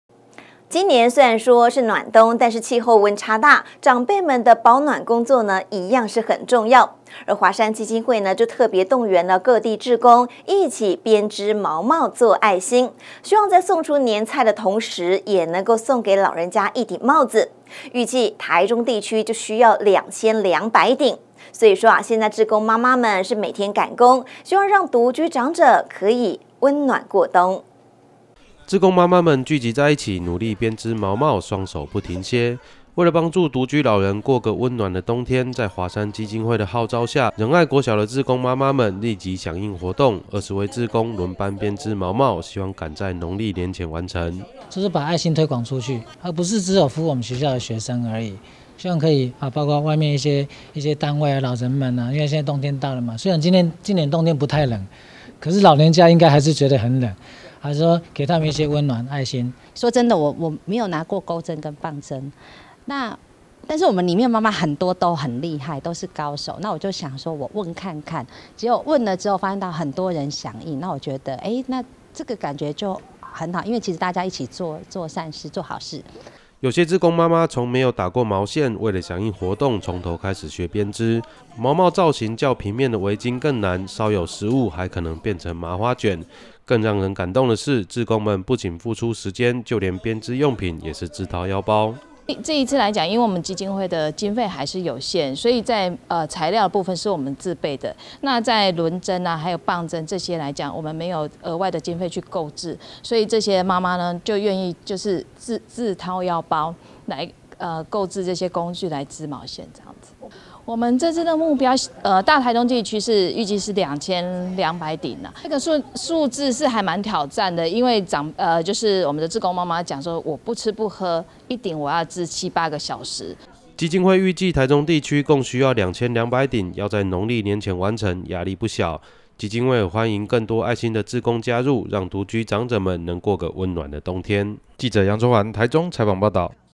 華山號召志工為獨老織毛帽
今年雖然是暖冬，但氣候溫差大，長輩們的保暖工作就相當重要，華山基金會特別動員各地志工，一起織毛帽做愛心，希望在送出年菜的同時，也能送給老人家一頂帽子，預計台中地區需要2200頂，現在志工媽媽們每天趕工，希望讓獨居長者可以溫暖過冬。